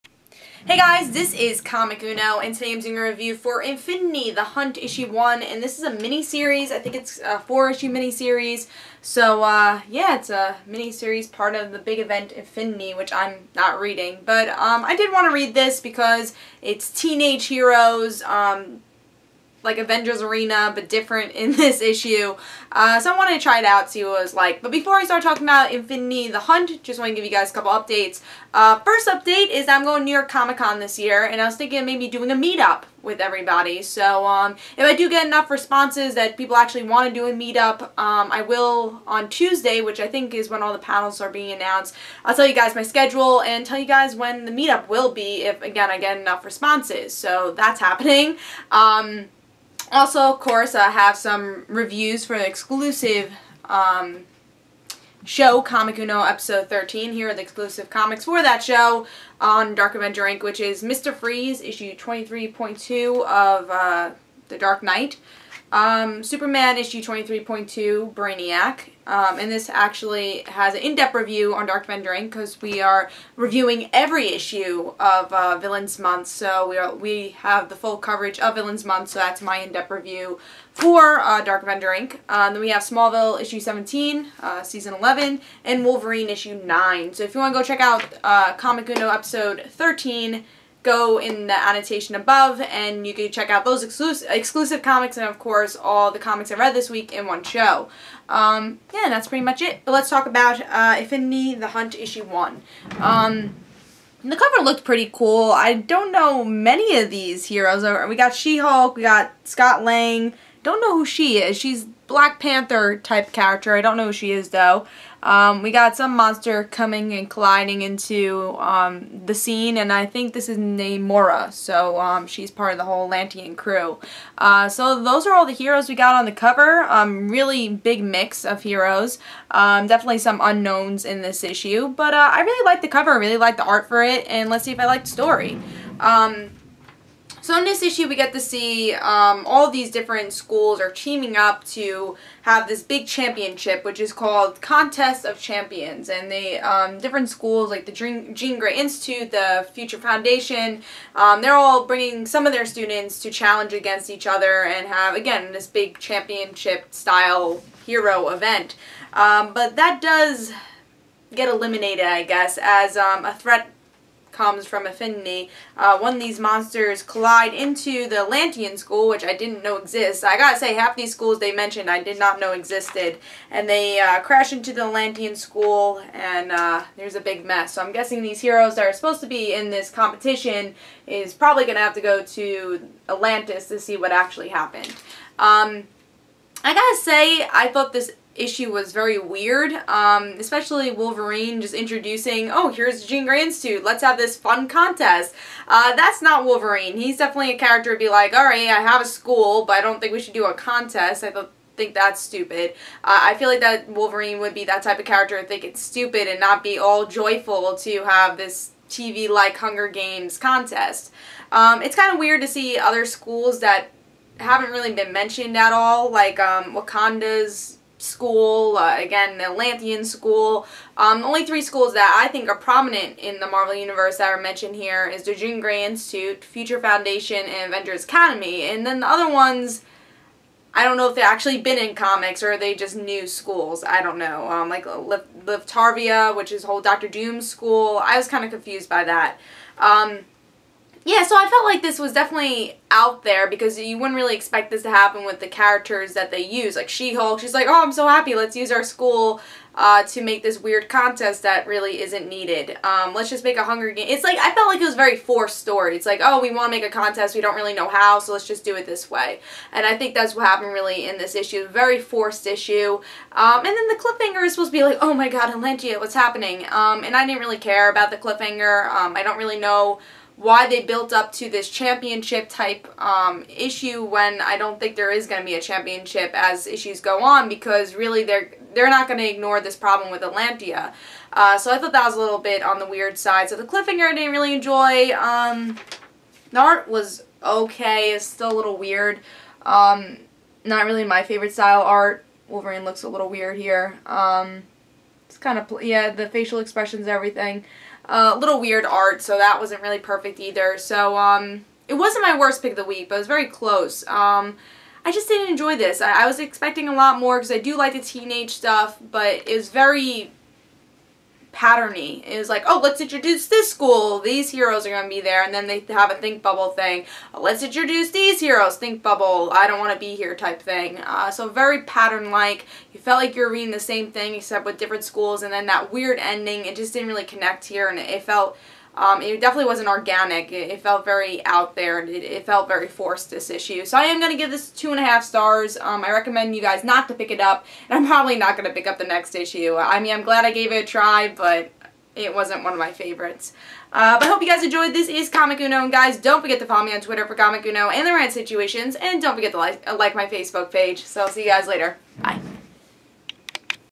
Comic Uno Infinity The Hunt #1 (review)
Here is my comic book review for Infinity The Hunt issue 1.